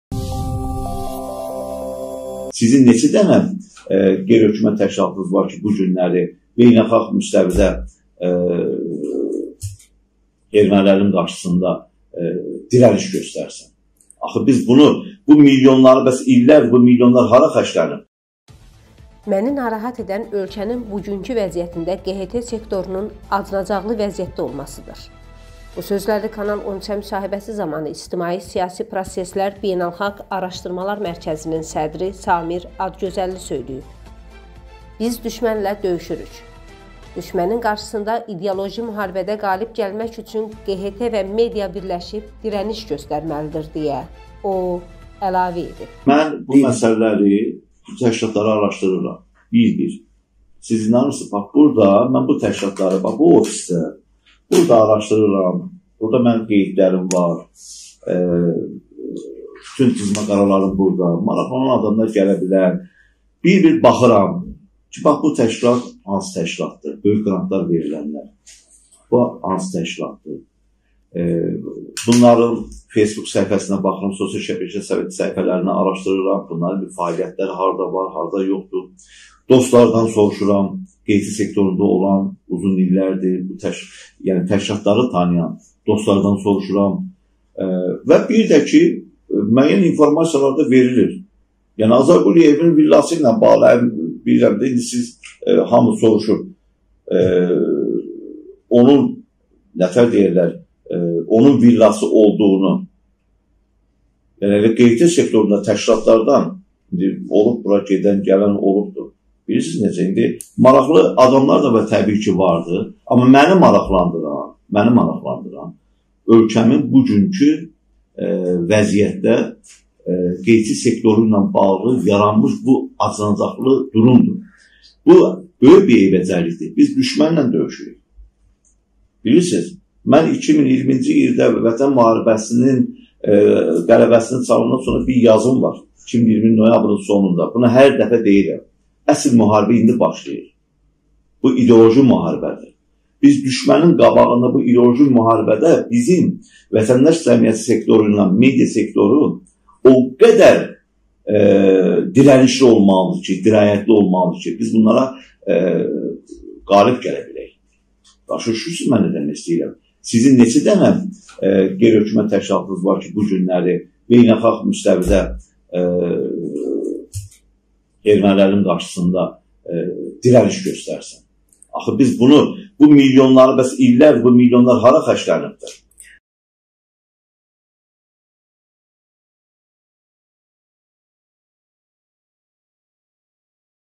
"Azay Quliyevin böyük villası, içərisində stadionu var, bu milyonlar..."-Samir Adıgözəlli
"Məni narahat edən ölkənin bu günkü vəziyyətində  QHT sektorunun acınacaqlı vəziyyətdə olmasıdır"

Bu sözləri Kanal 13-ə müsahibəsi zamanı İctimai Siyasi Proseslər Beynəlxalq Araşdırmalar Mərkəzinin sədri Samir  Adgözəlli söyləyib.

"Biz düşmənlə döyüşürük, düşmənin qarşısında ideoloji müharibədə qalib gəlmək üçün QHT və media birləşib dirəniş göstərməlidir"-deyə, o əlavə edib#kanal13abunəsiol ​ VideonuBəyən​